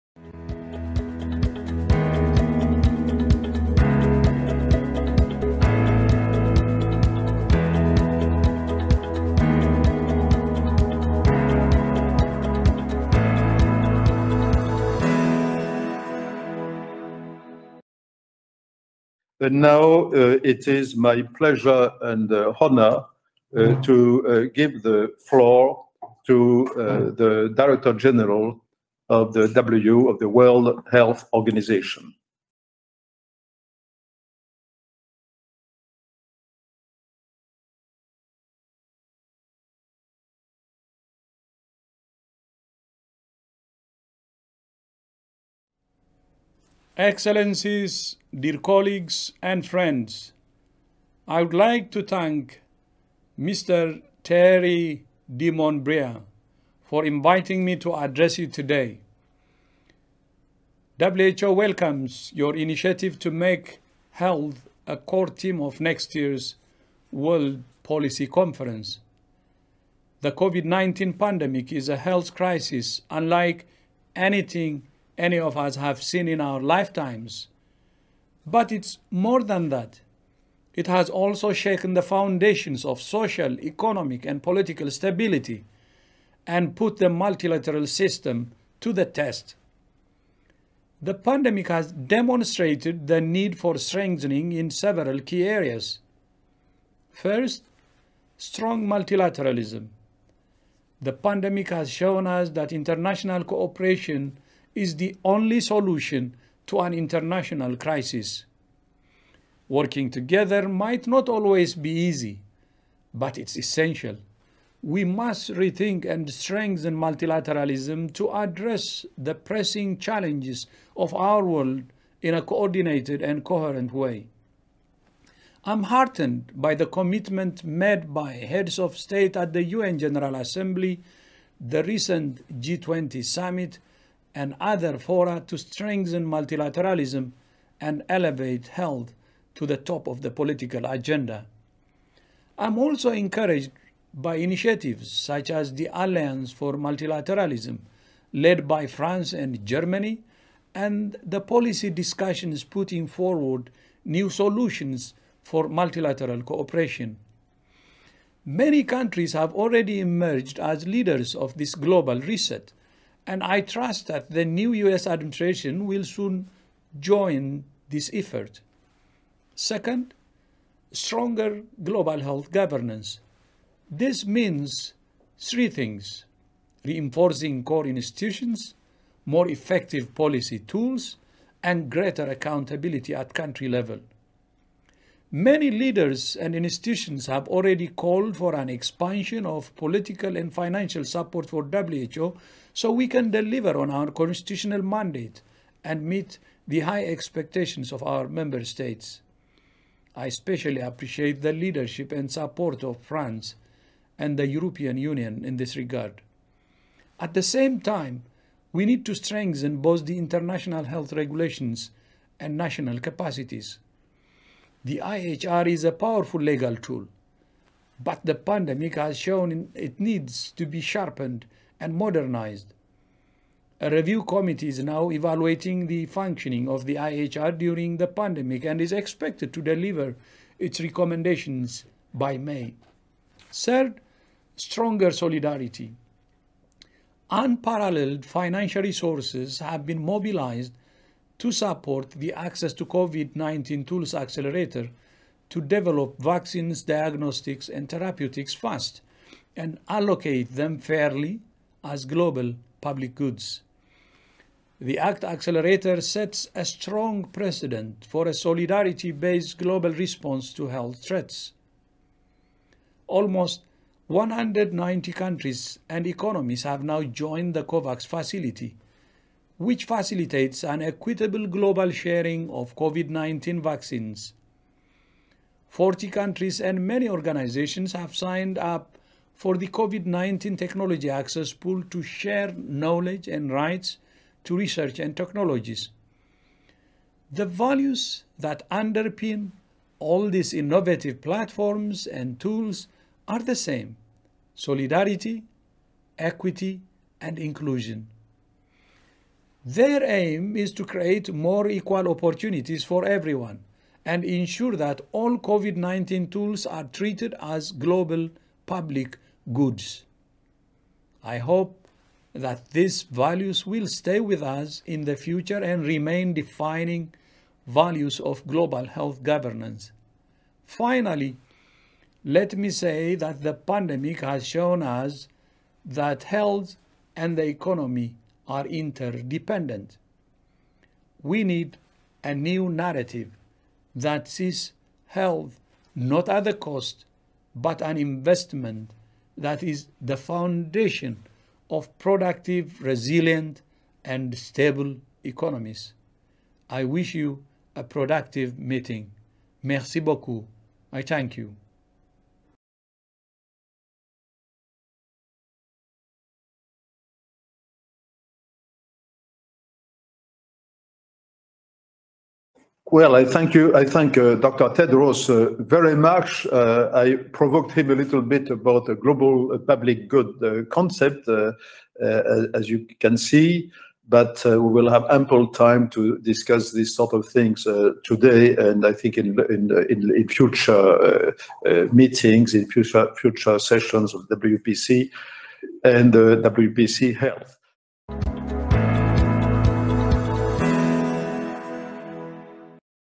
WPC – Health 2020 - Tedros Adhanom Ghebreyesus - Opening: Global Governance and Public Health
First edition of the WPC – Health • December 2, 2020 • Online meeting

Dr. Tedros Adhanom Ghebreyesus is the Director-General of the World Health Organization. He addressed a keynote speech at the WPC – Health.